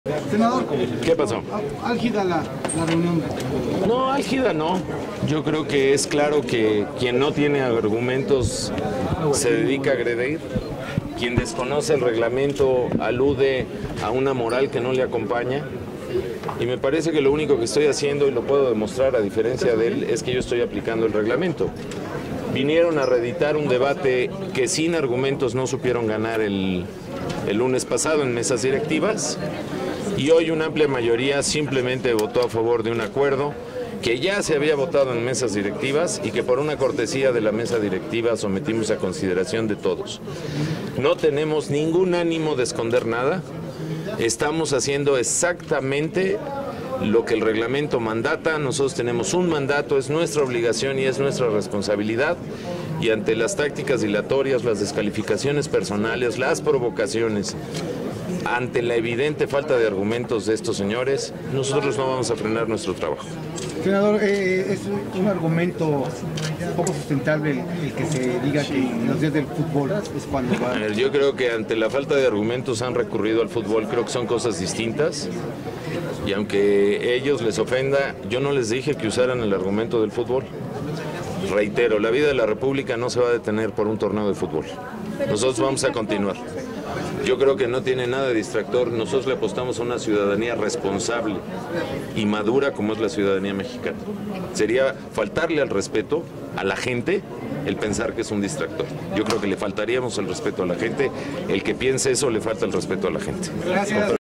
La vida de la República no se detendrá por un torneo de futbol: Senador Penchyna
Entrevista al Senador David Penchyna al finalizar la reunión de comisiones unidas de energía y estudios legislativos primera.
Quien afirma que el futbol es un distractor para la reforma energética, le falta el respeto a la gente